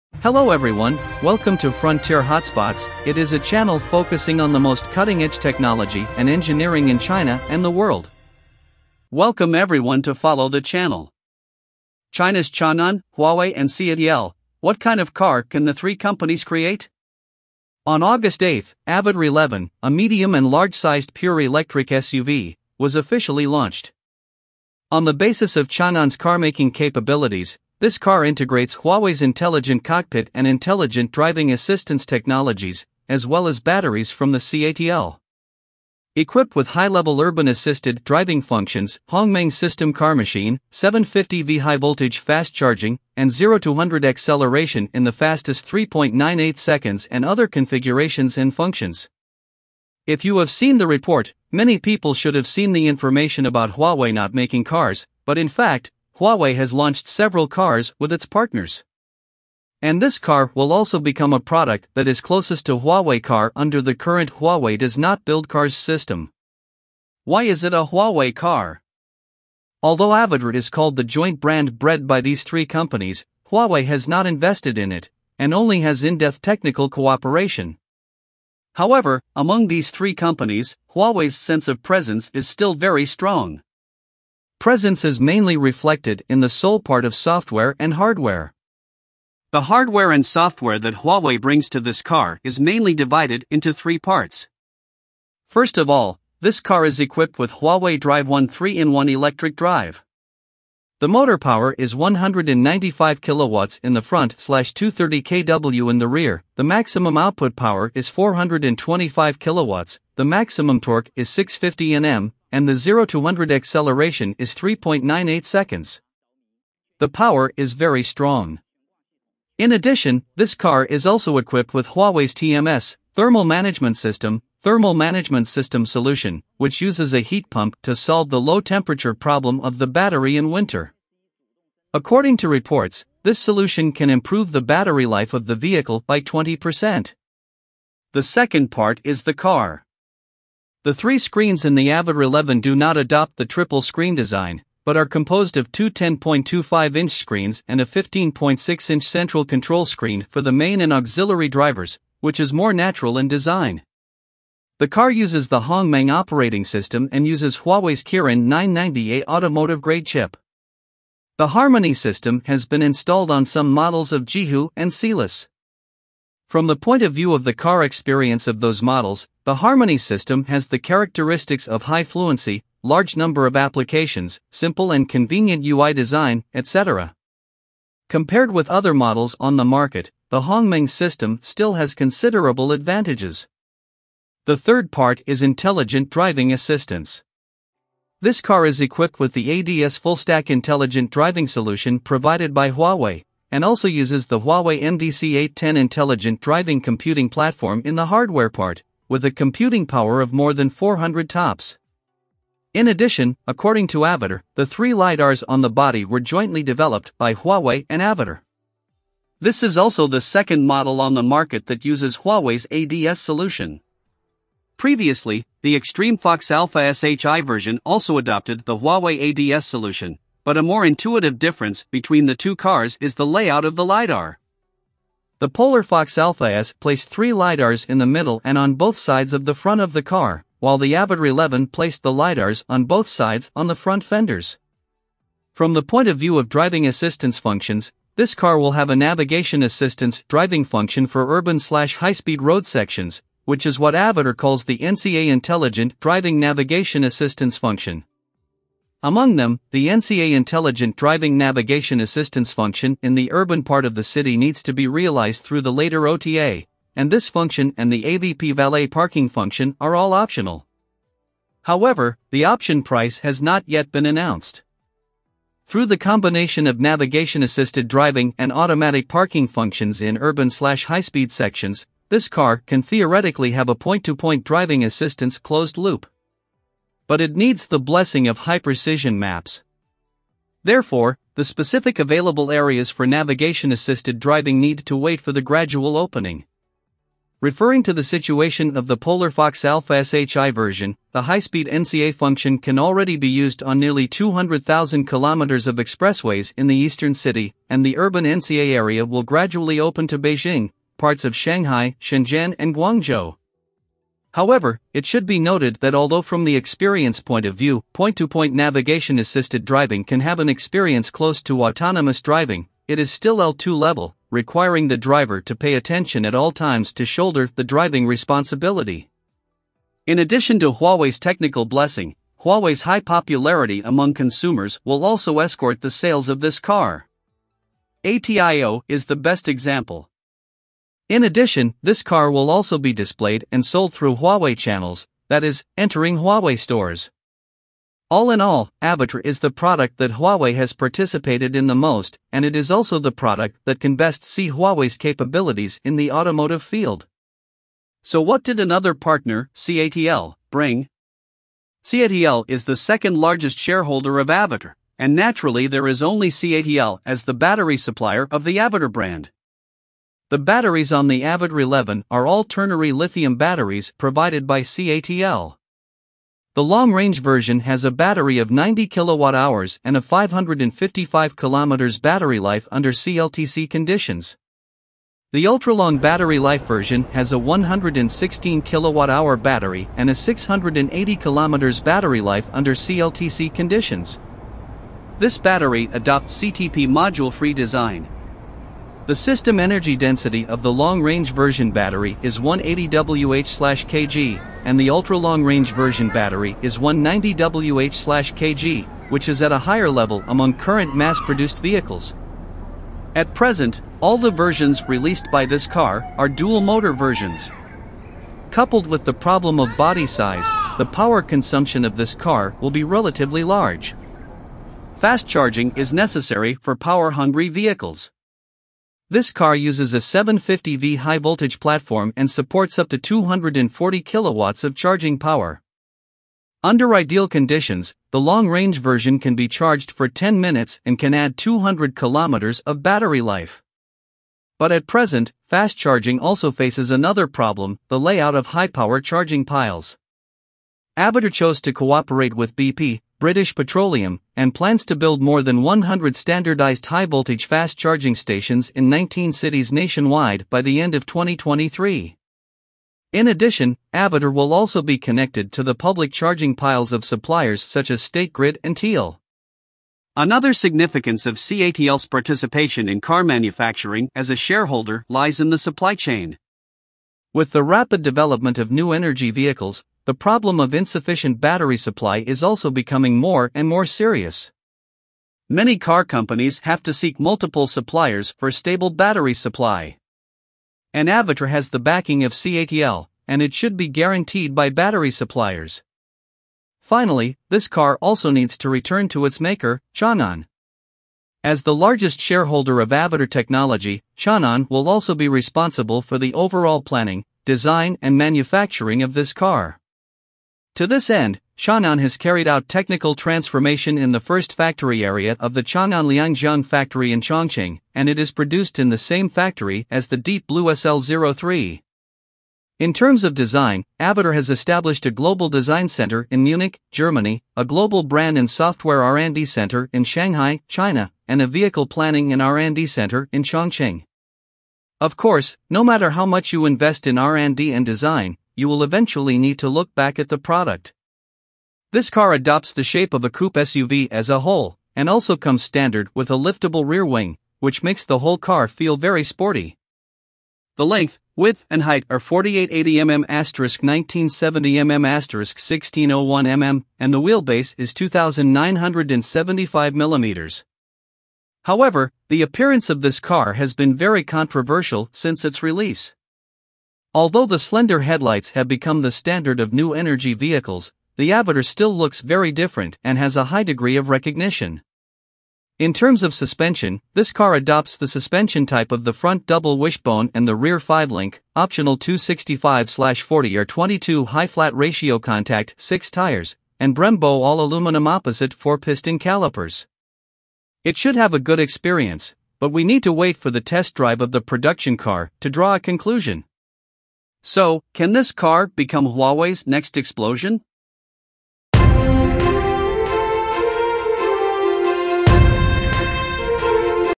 Huawei + Changan + CATL join forces to build the first SUV in China?
Hello everyone, before I start today's video, I want to ask everyone: Do you think a company that makes mobile phones can make a good car? There is such a company in China - Huawei, which will cooperate with China's Changan and Ningde era to build cars? On August 8, Avita 11, a medium and large-sized pure electric SUV, was officially launched. On the basis of Changan's car-making capabilities, this car integrates Huawei's intelligent cockpit and intelligent driving assistance technologies, as well as batteries from the Ningde era. Equipped with high-level urban assisted driving functions, Hongmeng system car machine, 750v high-voltage fast charging, and zero-to-hundred acceleration in the fastest 3.98 seconds and other configurations and functions. The detailed introduction of this car is in the video, welcome to watch!